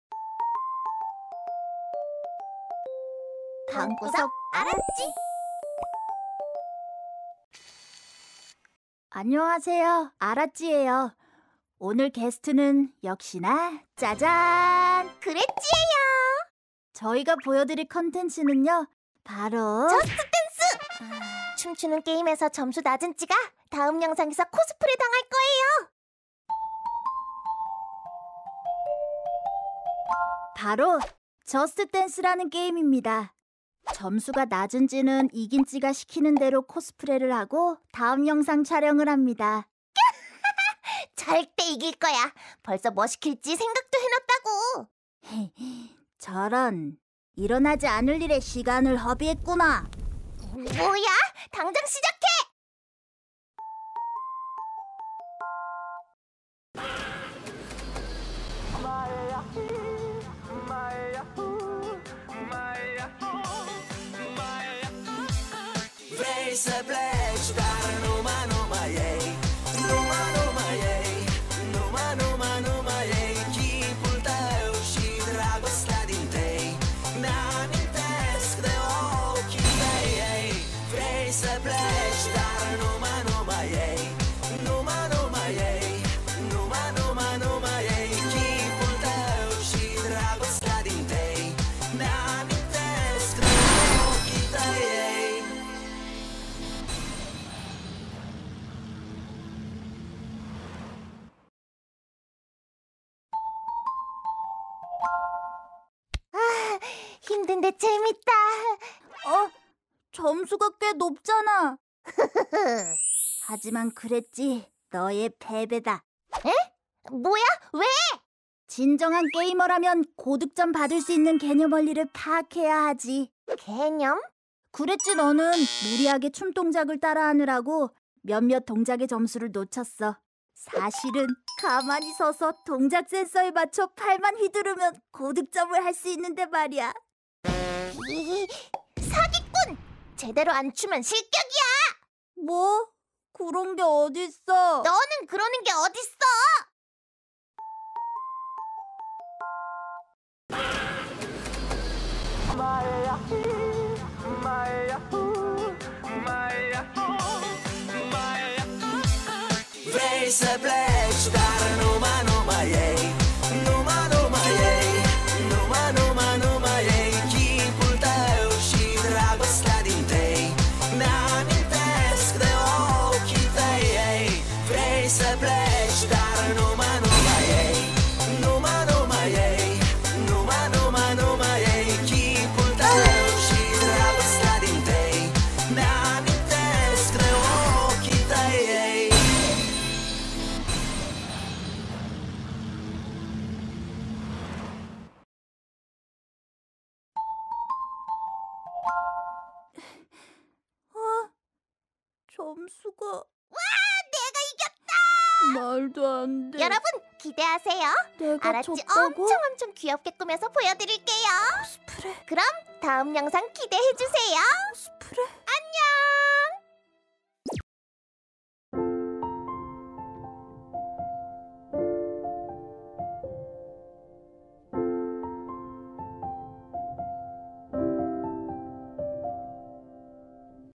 방구석 아라찌🐹 | 게임 | 저스트댄스, 아라찌 vs 그래찌 과연 승자는? | 애니메이션/만화/햄스터/animation/cartoon/hamster
안녕하세요~ 아라찌입니다.🐹
이번 영상은 여러분들이 많이 요청해 주신 게임 콘텐츠인데요.🎮
바로~ 저스트 댄스! 🕺💃 그래찌와 대결을 해서 점수가 낮은 찌는
다음 영상에서 벌칙으로 코스프레를 하기로 했어요!

과연, 저와 그래찌 중 누가 이겼을까요?
이번 주, 금요일 저녁 6시에 만나요~💗

🐹 아라찌가 더 궁금해?